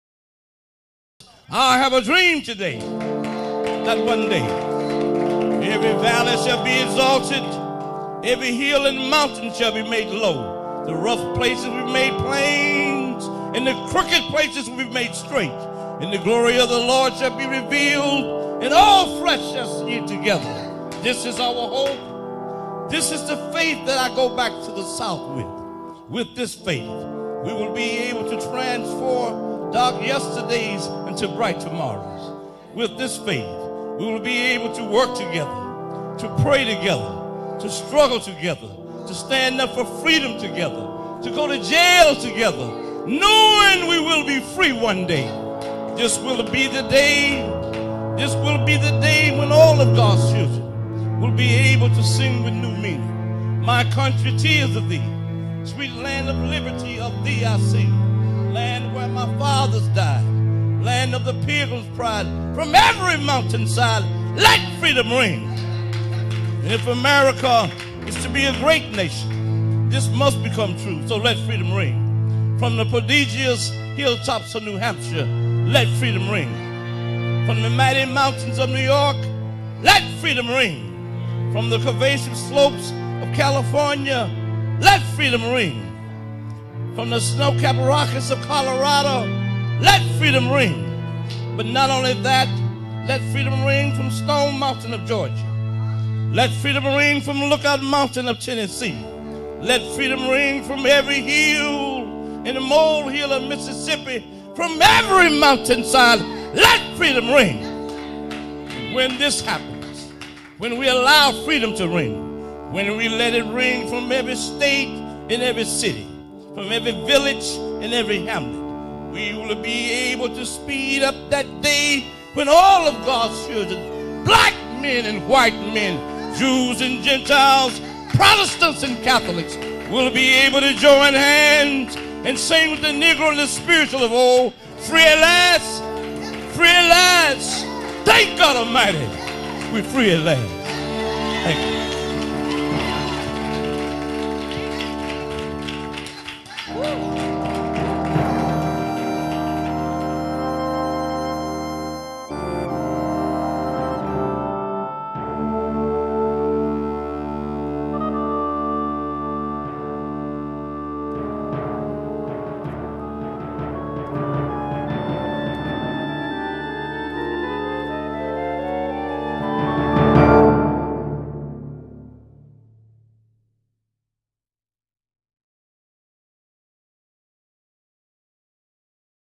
MLK DAY
My tribute to a man I hold in high regard.

The speaker is not MLK; but an orator who does a fine job reciting the "I Have A Dream" speech. Let me know what MLK meant to you.